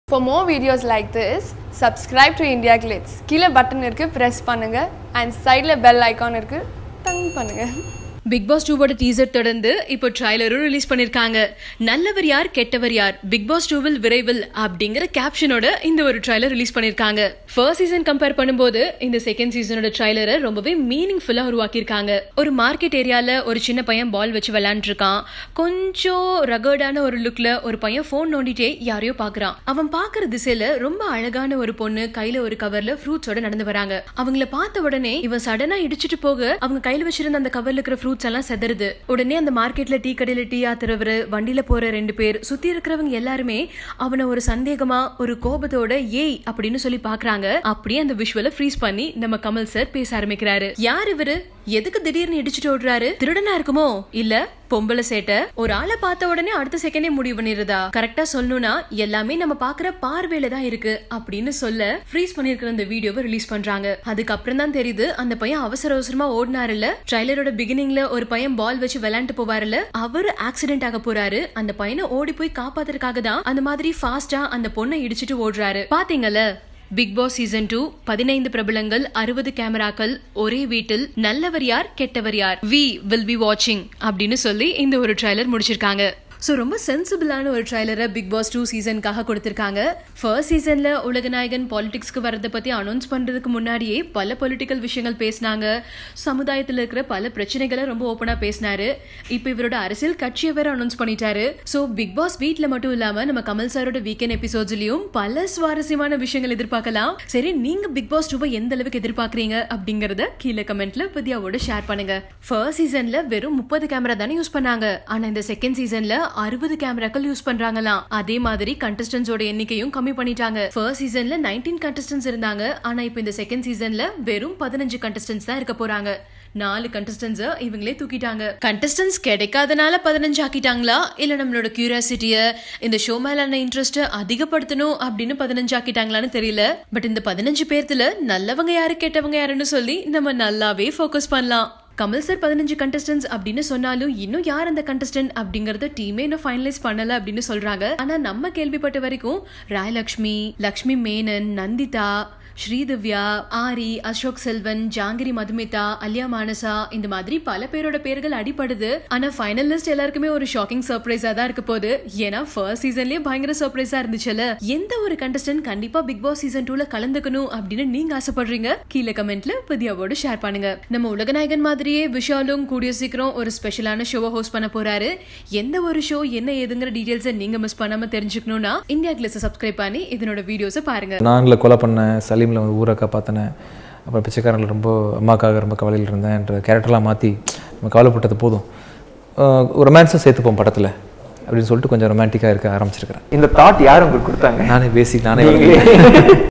BIGG BOSS 2 New Trailer Review | Kamal Hassan | Vijay TV Tamil
Bigg Boss Tamil season 2 new promo: Kamal Haasan lets a thief run away, but there is a twist